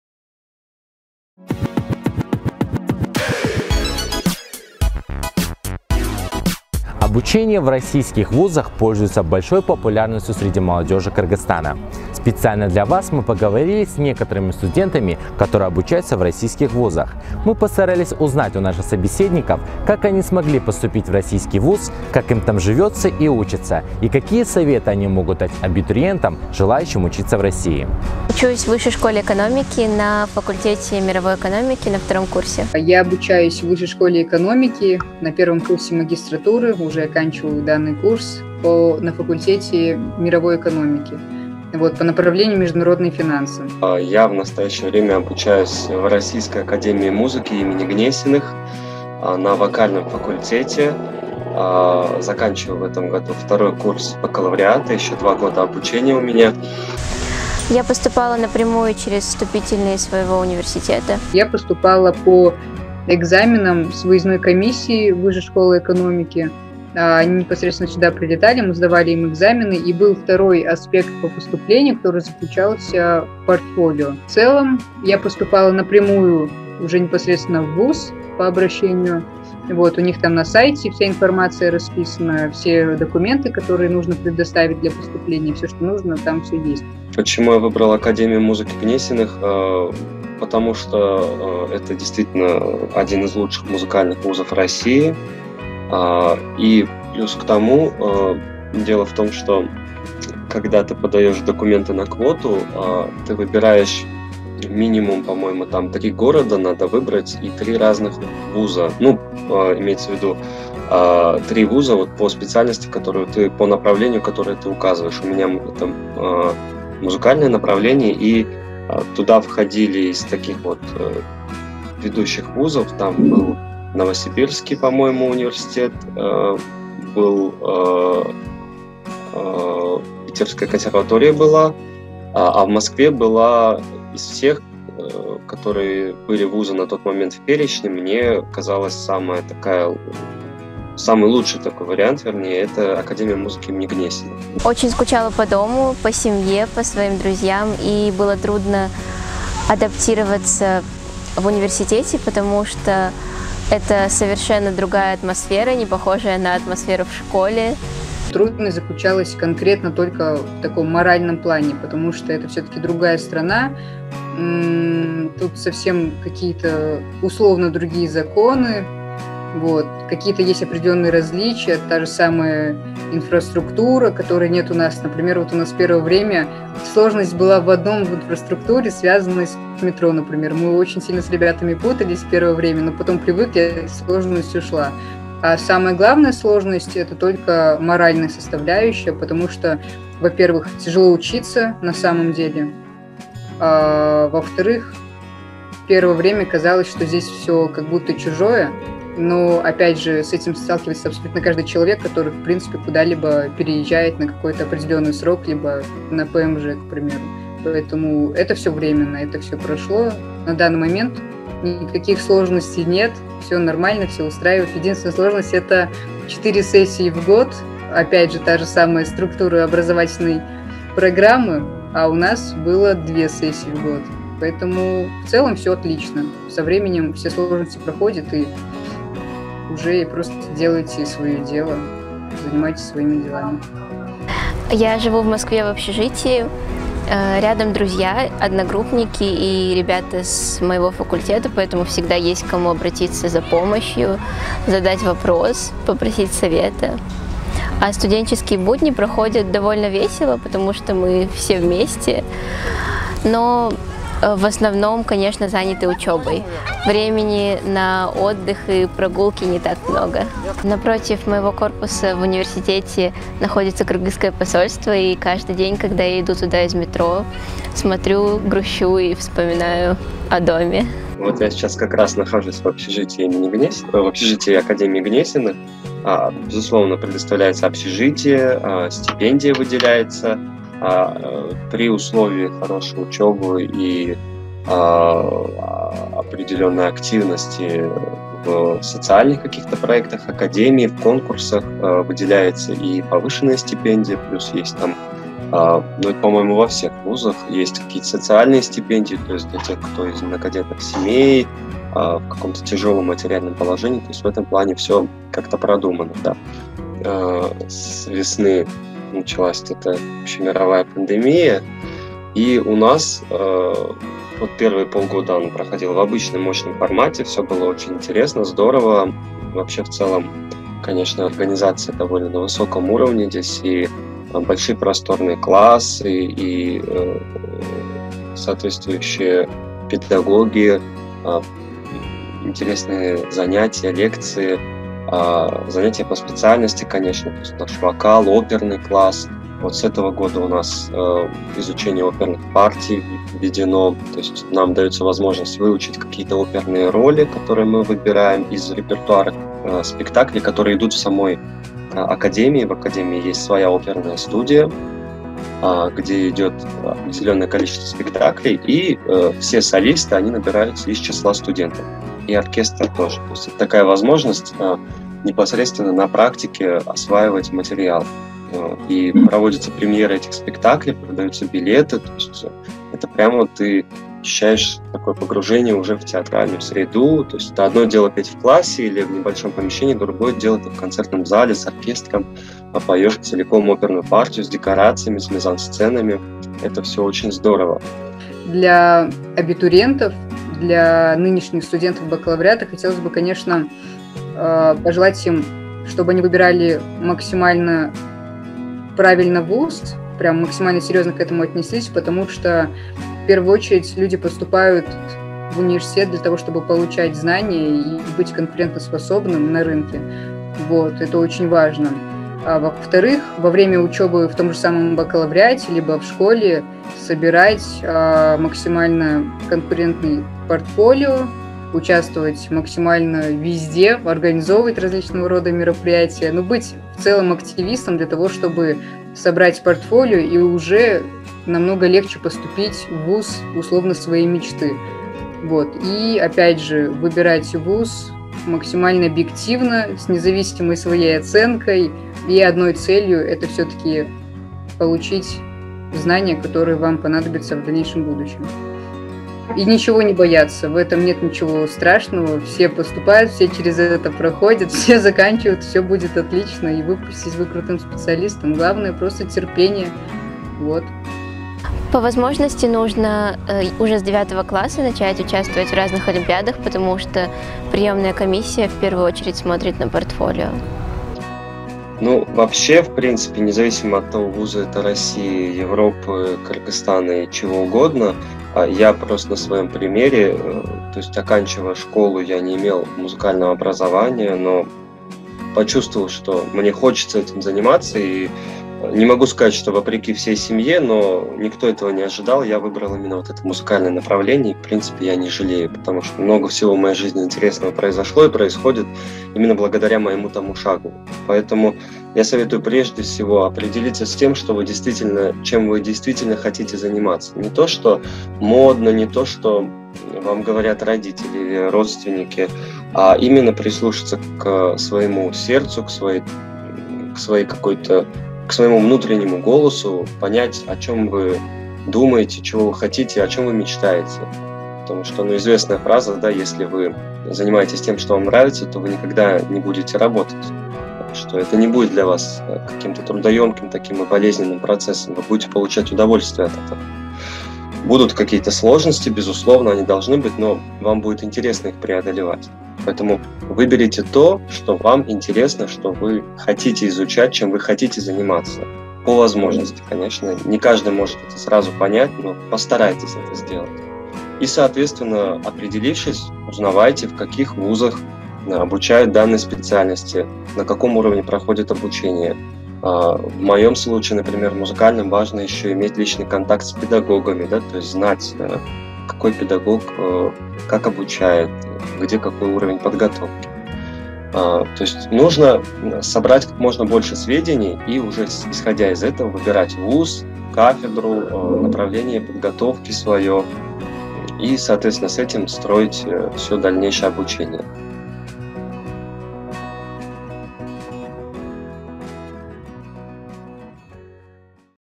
Сложно ли кыргызстанцам учиться в ВУЗах России
Российские вузы пользуются большой популярностью среди молодежи Кыргызстана.
Сегодня мы поговорили со студентами, которые в данный момент обучаются в РФ, для того, чтобы получше узнать: как они поступили в российские вузы, с какими проблемами столкнулись и какие они дали бы советы абитуриентам.